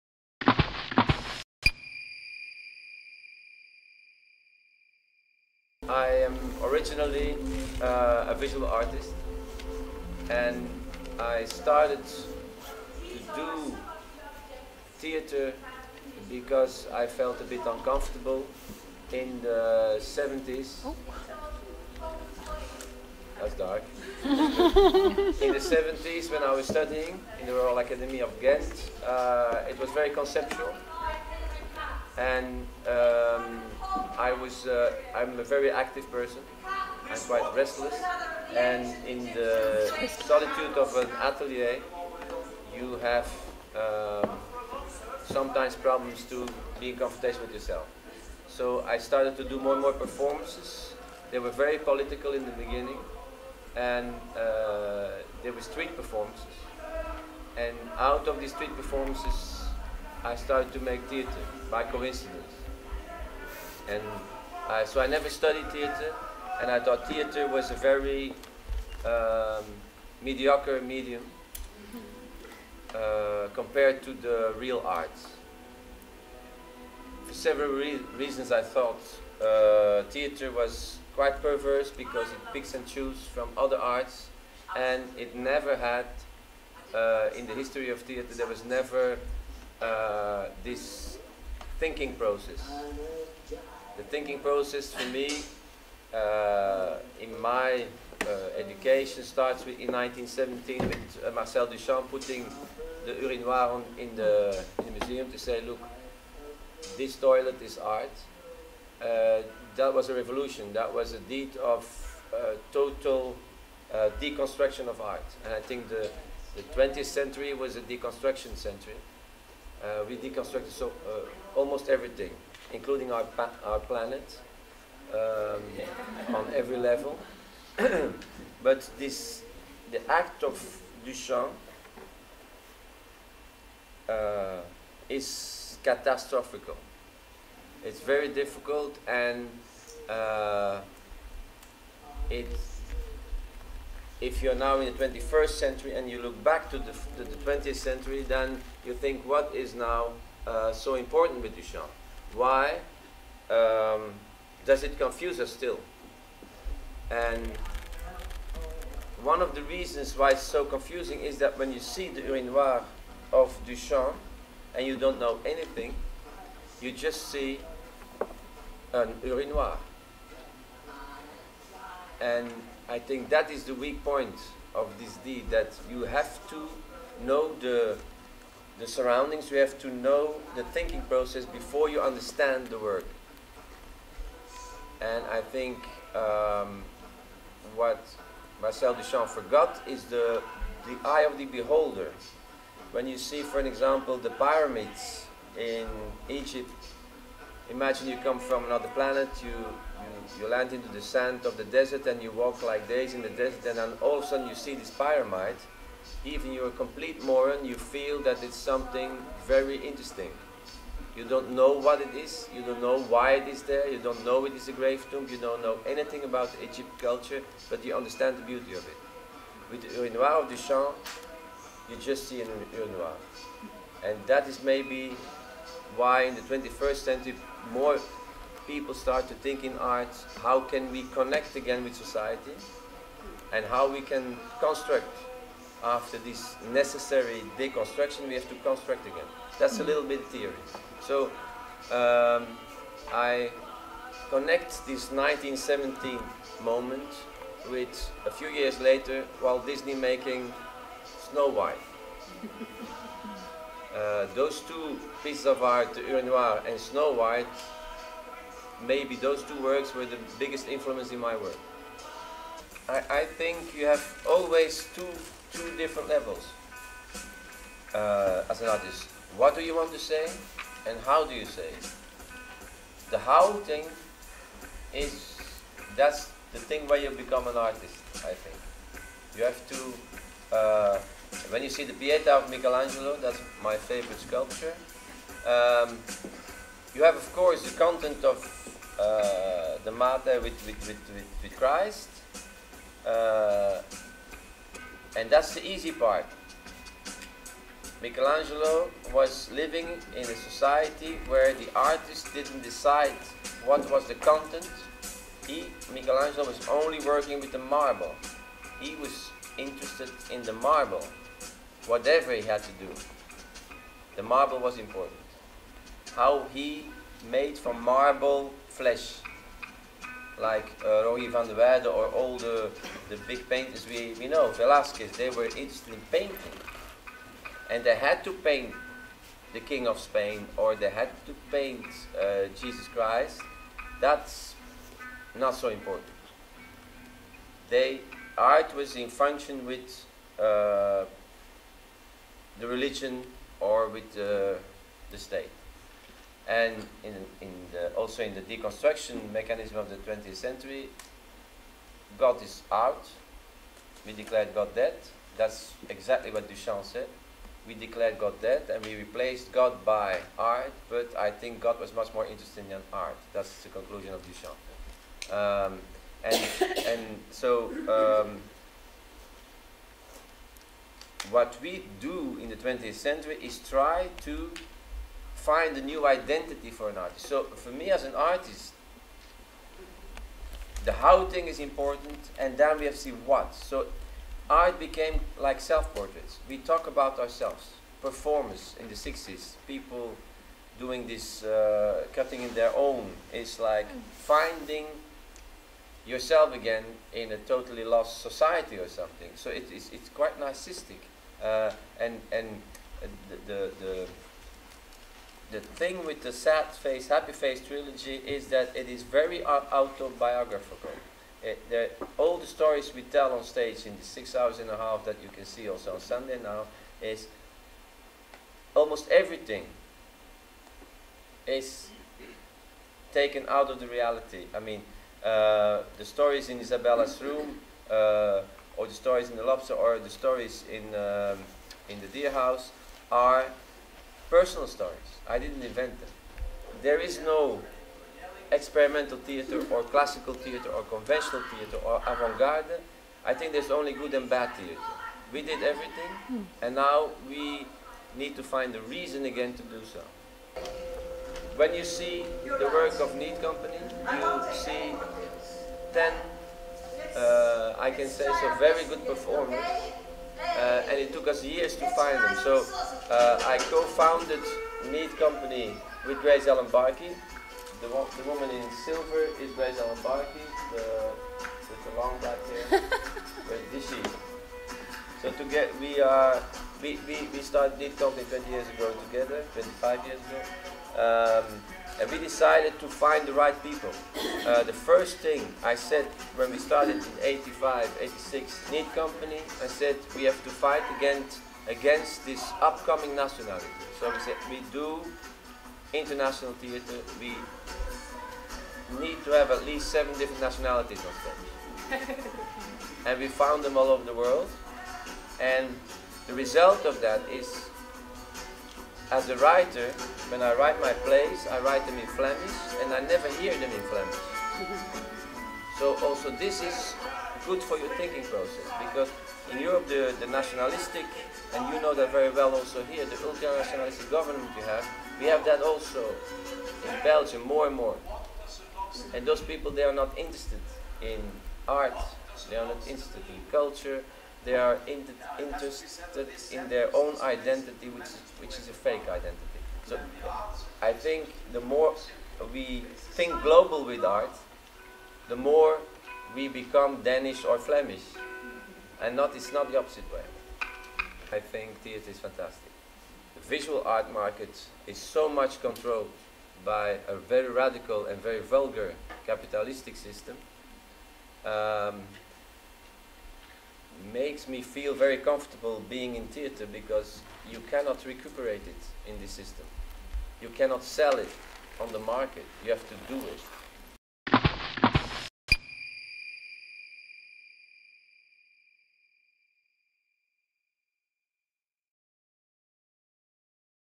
JAN LAUWERS PART I - BARBARATORIUM
Jan Lauwers (Antwerpen, 1957) er en kunstner, der arbejder i stort set alle medie. I løbet af de sidste tyve år har han blevet bedst kendt for sit banebrydende arbejde for scenen med Needcompany, som blev grundlagt i Bruxelles i 1986. I årenes løb har han også opbygget en betydelig samling af kunstværker, som blev vist i en udstilling på BOZAR (Bruxelles) i 2007.
Studerede malerkunst på Kunstakademiet i Gent.